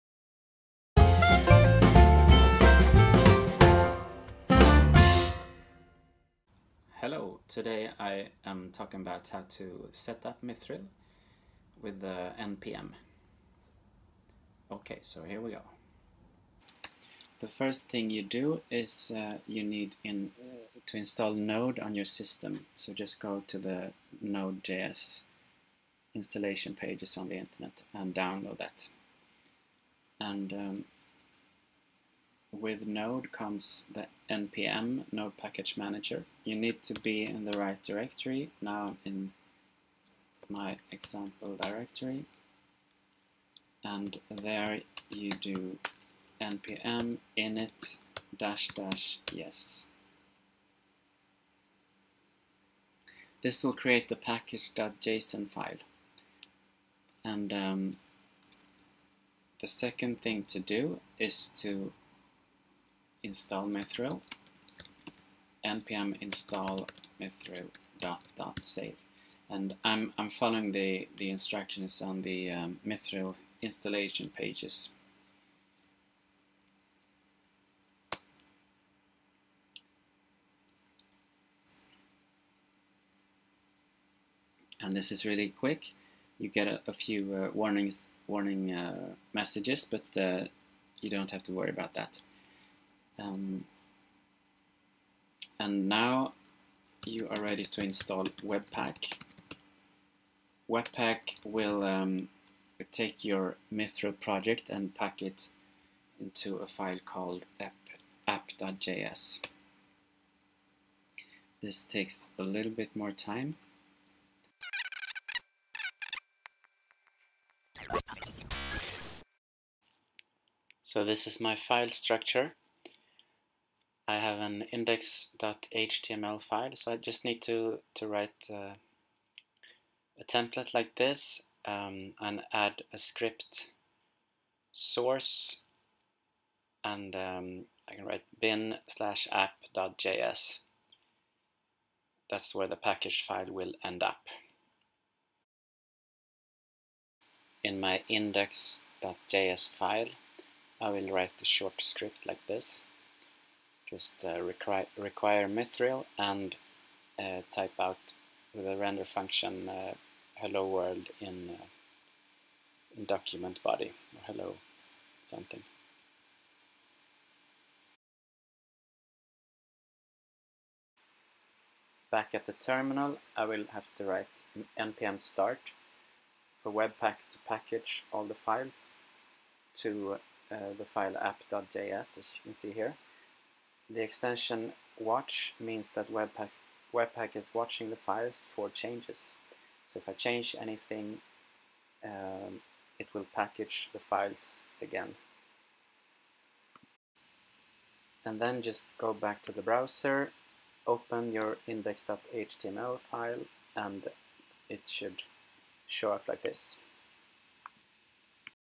Mithril.js – Installing
(updated with new webpack configuration)

Demonstration on how to set up and install the javascript mvc framework mithril.js, using npm and webpack.

Mithril js is a good start if you want to learn a javascript mvc framework, and has some similarities with react js, in that it uses a virtual dom, views and components.

Sorry for the bad sound in parts of the video!